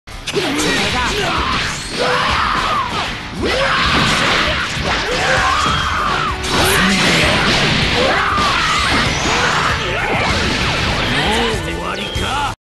Kinda useless rejump tag in combo with Gogeta4 from UI 5M.
Title says it all. Did on Broly cause I didn’t have much time and I wanted to do it easy.